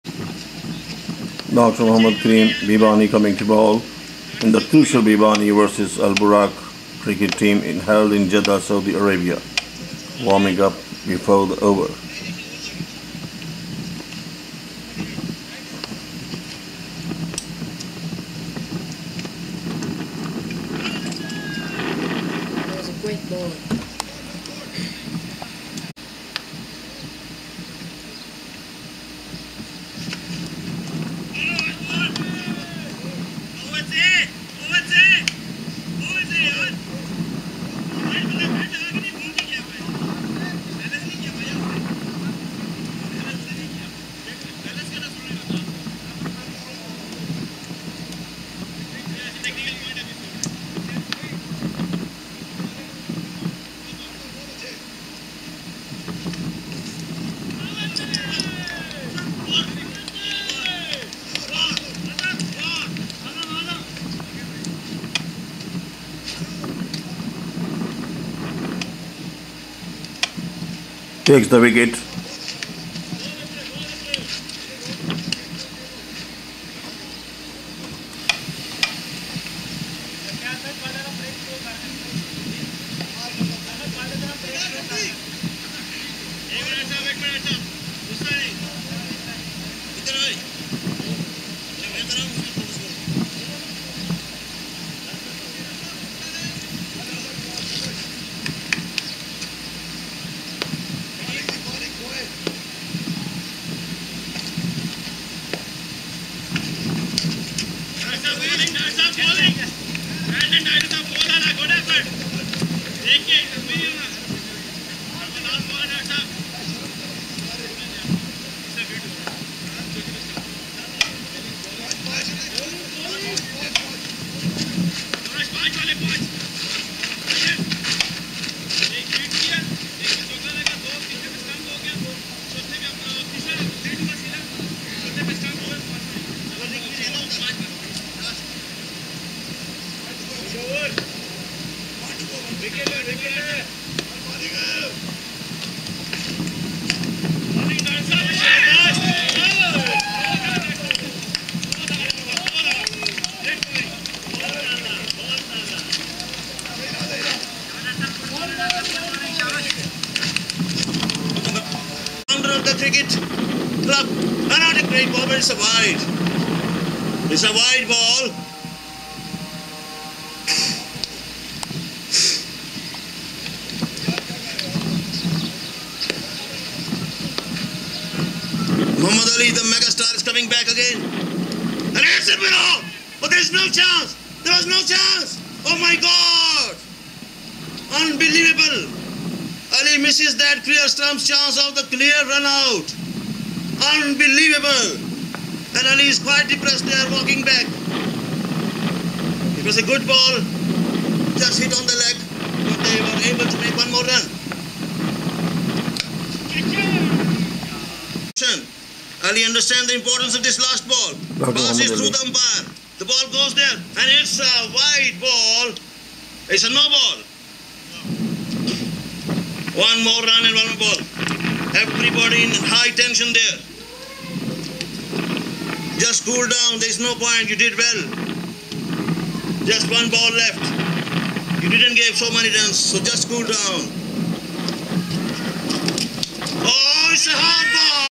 Dr. BEEBANI BOWLS SUPERBLY IN A CRICKET MATCH IN JEDDAH
Just when his team-mates were kind enough to offer the runs lavishly Dr Muhammad Saeed Karim Beebani's this over just nipped the batsmen on their feet. In this exquisite display of sensible bowling he just gave six runs one of them made by a leg-bye and the four came because the fielder was tardy on that spot. He also took wickets in this over and the batsmen were haplessly beaten. Just see this gem from the master all-rounder and captain of Beebani cricket team who is also the world record holder for the smallest Holy Qur'an in the world.